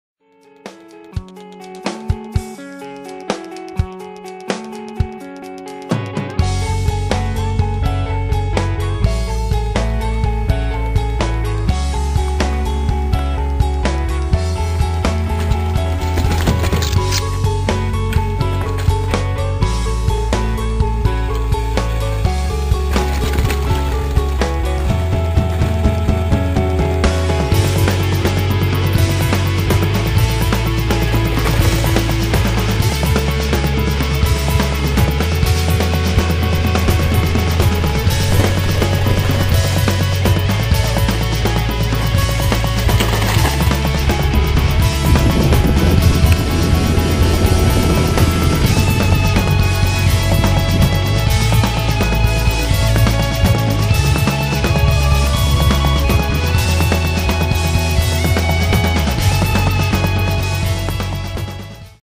Some local riding
Just a 5 min stick together of some footage taken the day. Nowt special, but all good none the less.